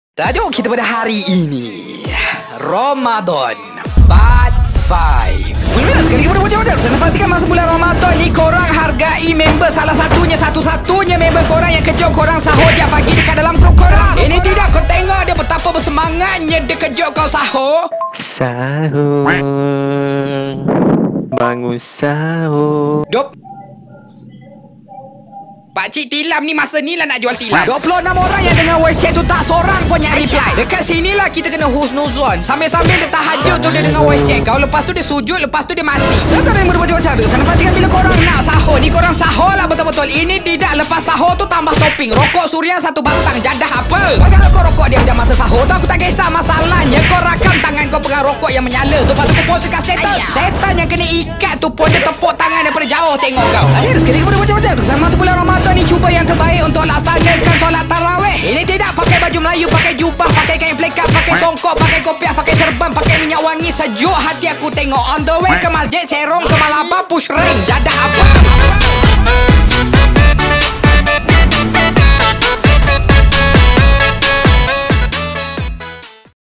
Ariq asyik spam mesej time sahur | Animation Bulan Ramadhan Part 5
[DON'T TRACE/COPY/STEAL MY ARTWORK AND I DON'T ALLOW ANYONE TO REPOST MY VIDEO ON ANY SOCIAL MEDIA]
Feat:
Aqil 💅
Ariq
Member lain
Kucing

Apps:
Flipaclip
IbisPaint X
Capcut

Take Time:1 months ago

Comic on Instagram:@x_.syaz._x
Wattpad@justimilia